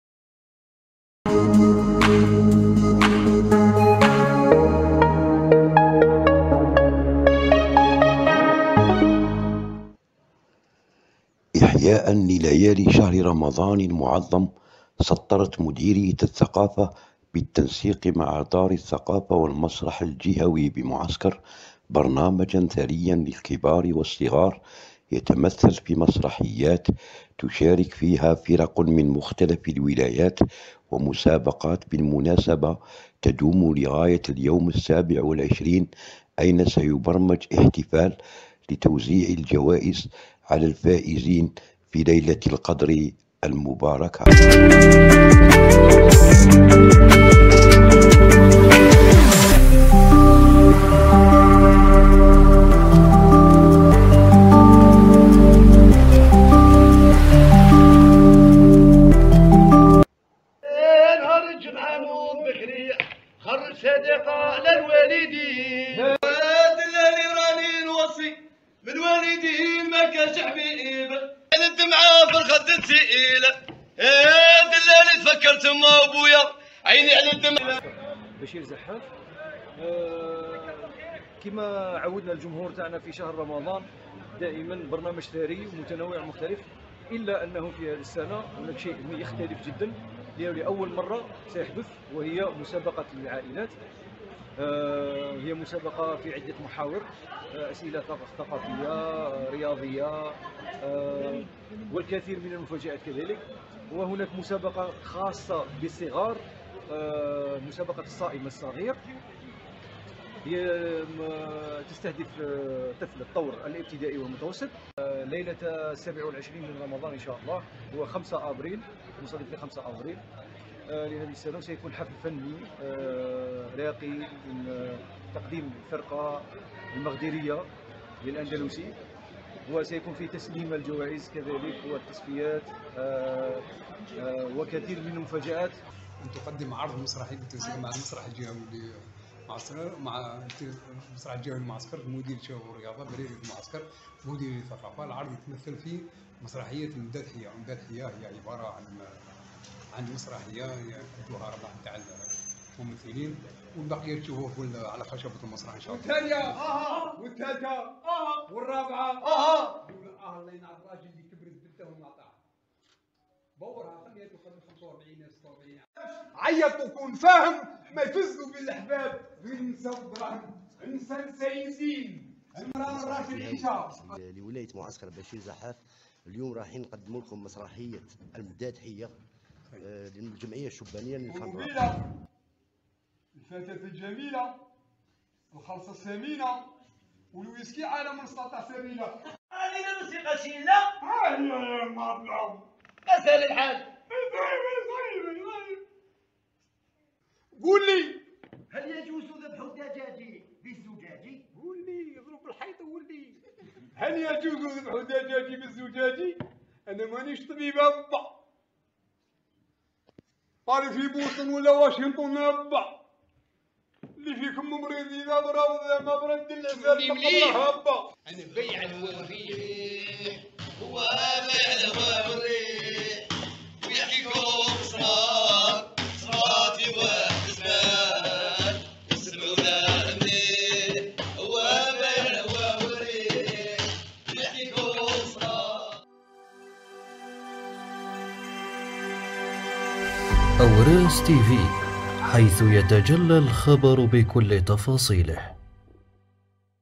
معسكر احياء لليالي شهر رمضان المعظمعرض مسرحية المدادحية بالمسرح الجهوي
معسكر احياء لليالي شهر رمضان المعظم
عرض مسرحية المدادحية بالمسرح الجهوي
تغطية سلطاني مختار